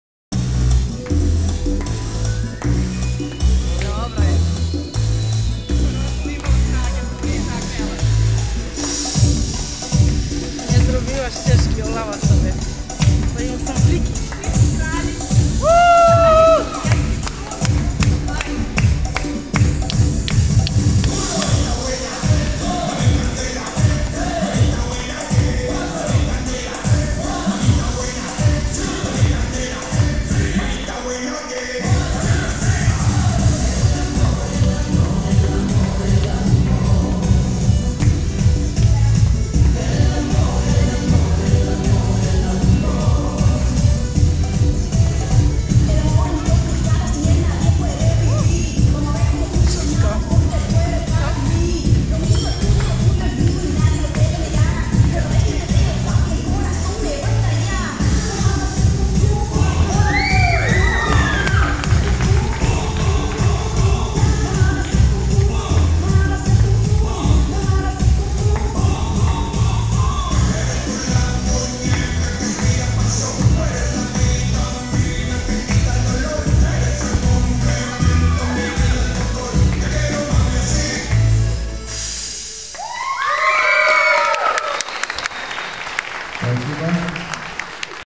Mistrzostwa Europyi Fitness FIT KIDs & Aerobic 2014 TALLIN Estonia -solo Wiktoria Parkot
Mistrzostwa Eeropy Fitnss TALLINN ESTONIA 2014
 wiktoria parkot